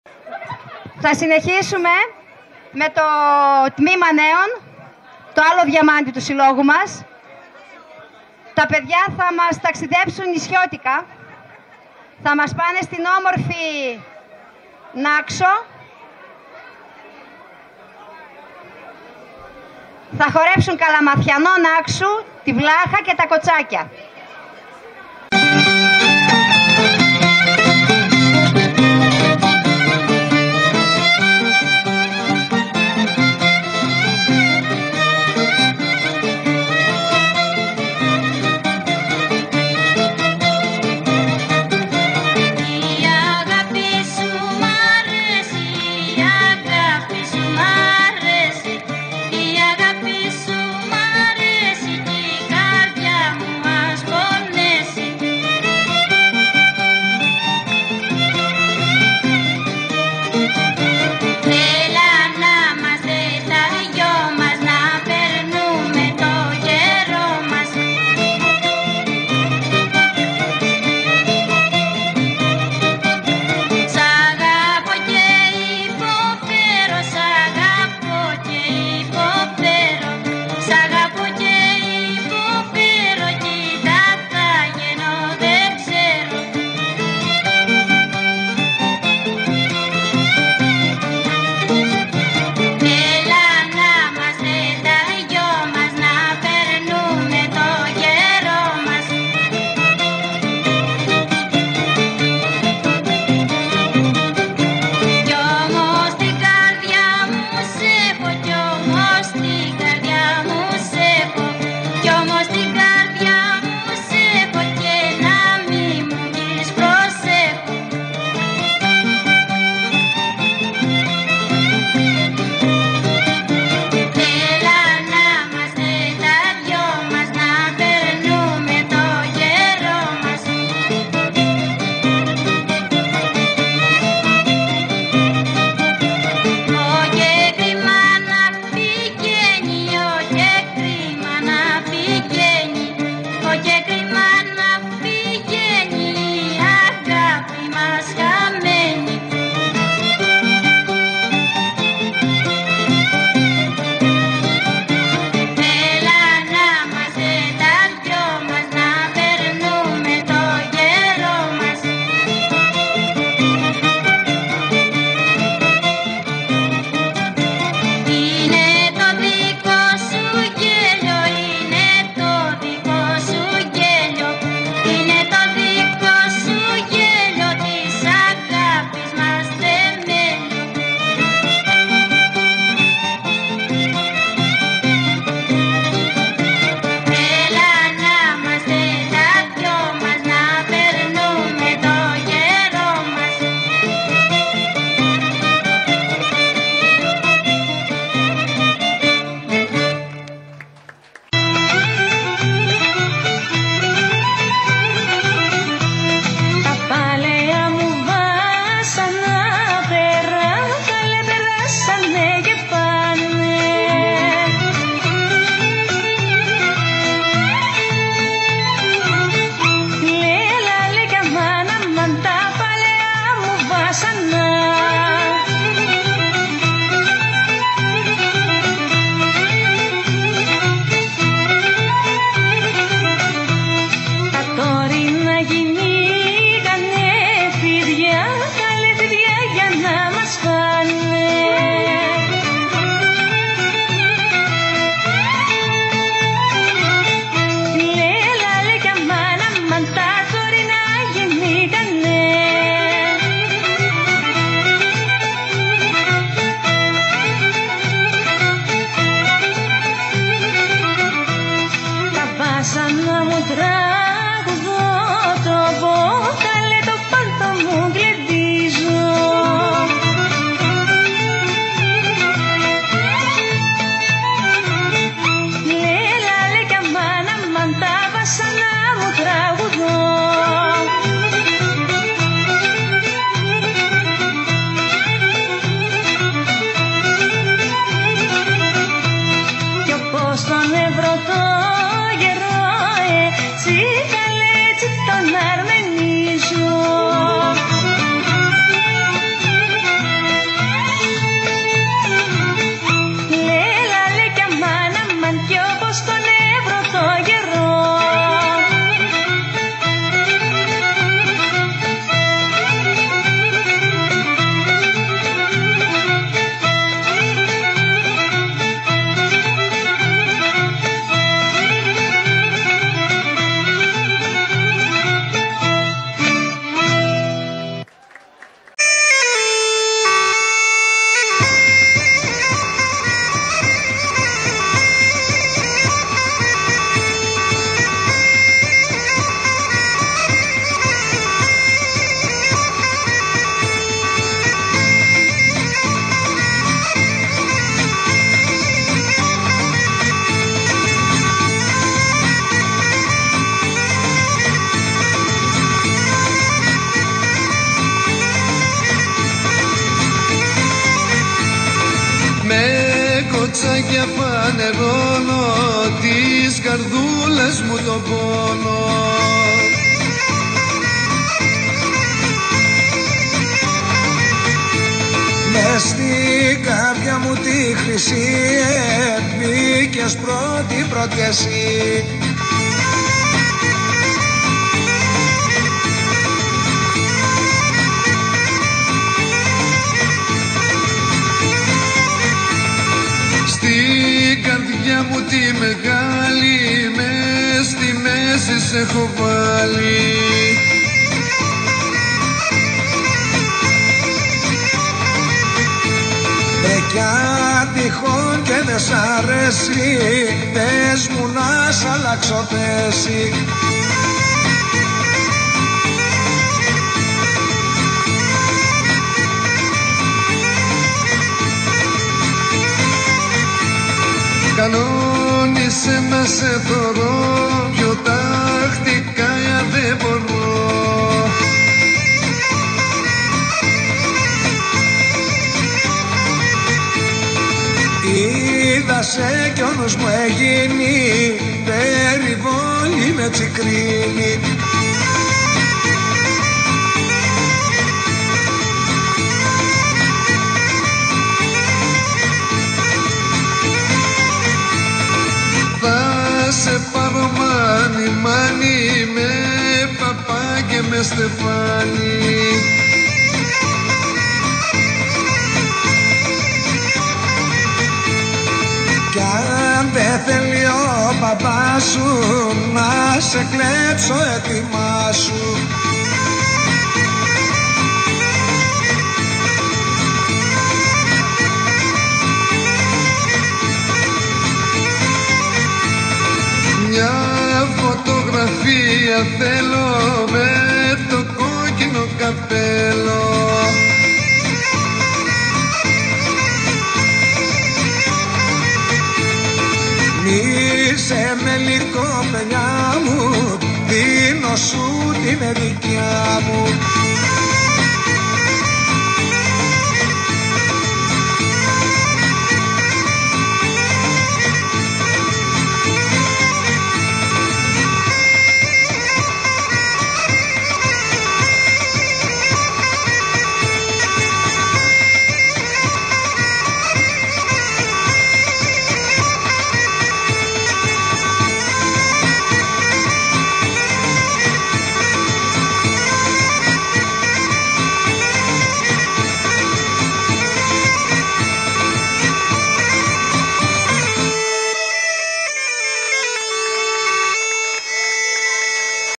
ΚΑΛΑΜΑΤΙΑΝΟΣ-ΒΛΑΧΑ-ΚΟΤΣΑΚΙΑ (Νάξου)-Τμήμα ΝΕΩΝ-ΓΛΕΝΤΙ ΚΟΠΗΣ ΒΑΣΙΛΟΠΙΤΑΣ ΧΟΡΕΥΤΙΚΗΣ ΑΚΑΔΗΜΙΑΣ ΠΕΥΚΩΝ
Στην κατάμεστη απο φίλους -μέλη-χορευτές και εκπροσώπους της δημοτικής αρχής αίθουσα της ταβέρνας "Κορυφή" στα Πευκα έκοψε την Βασιλόπιτα η
Στην συνέχεια ακολούθησε ένα τρικούβερτο_γλέντι που ξεκίνησαν κάποια τμήματα της Ακαδημίας εν είδη γλεντιού και όχι παράστασης .
Χοροδιδάσκαλος :Μάχη Βελώνη